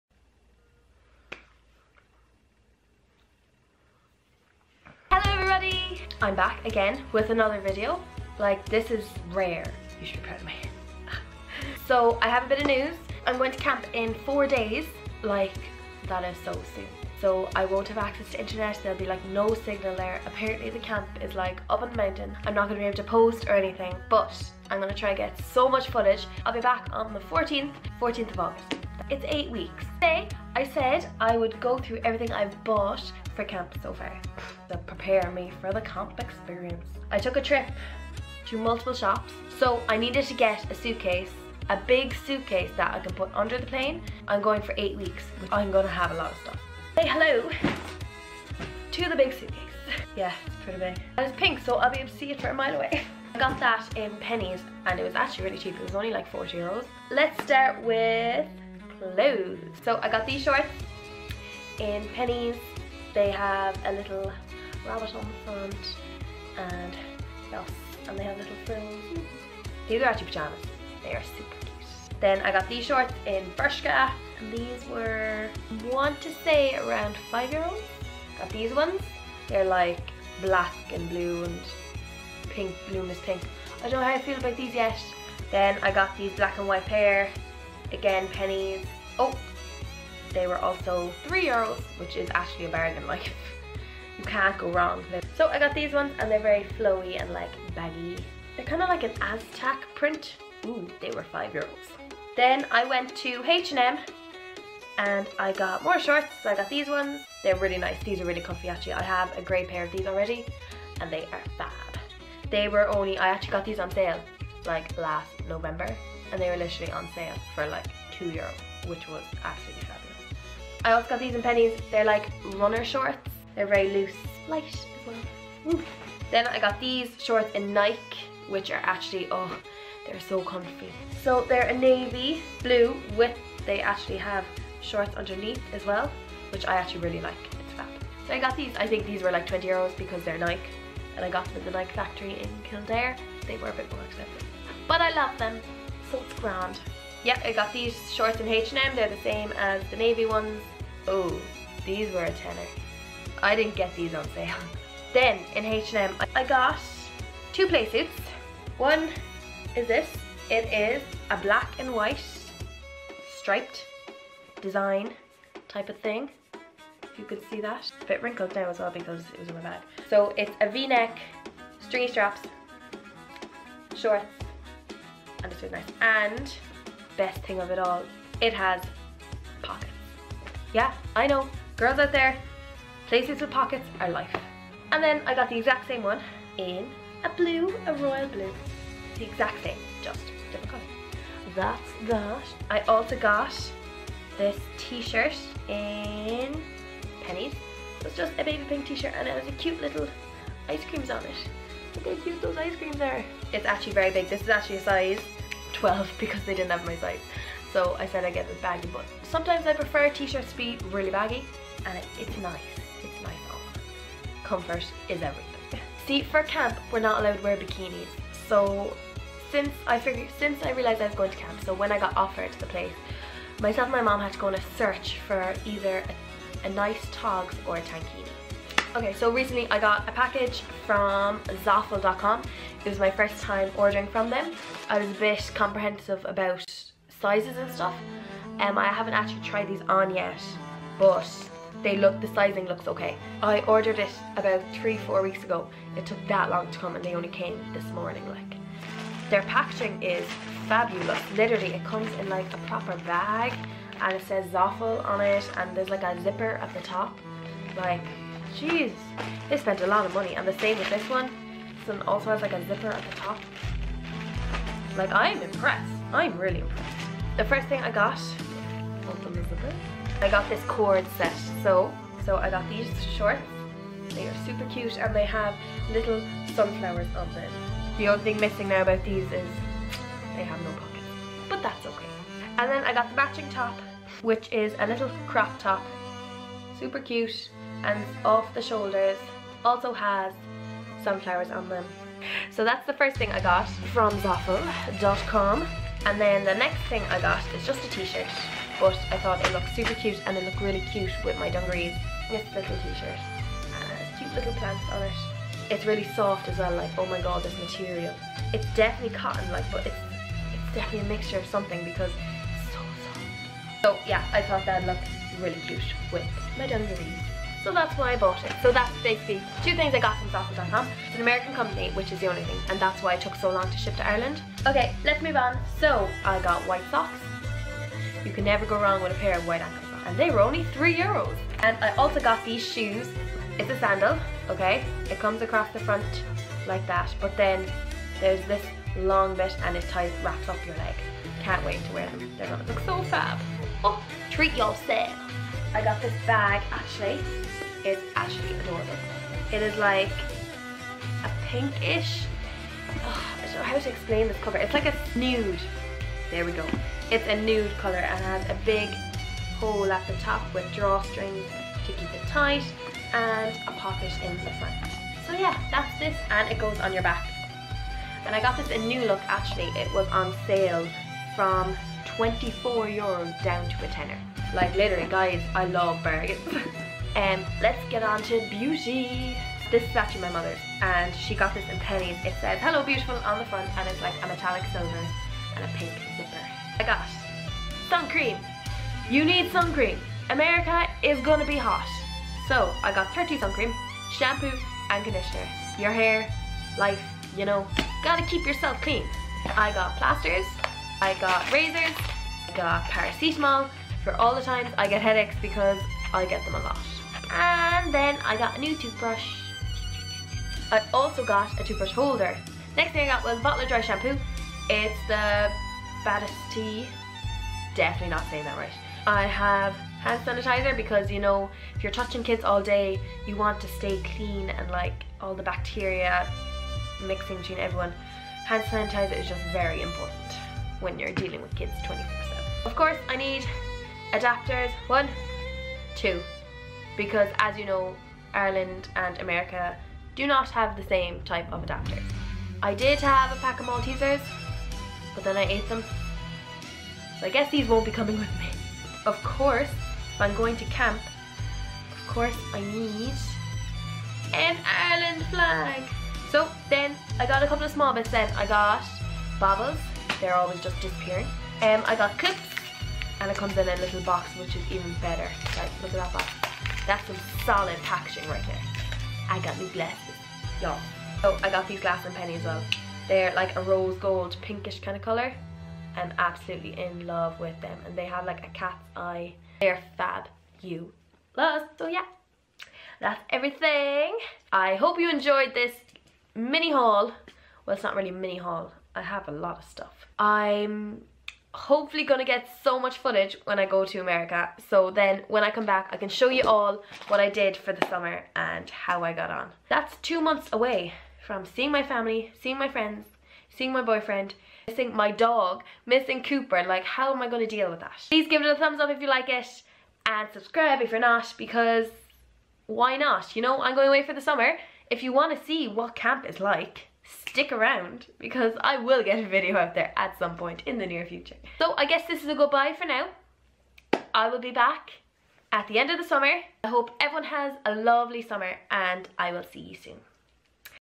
Camp Haul and a bit of News
Everything I bought to prepare for my trip to camp.
See ye soon xx

Hope everyone has a great Summer!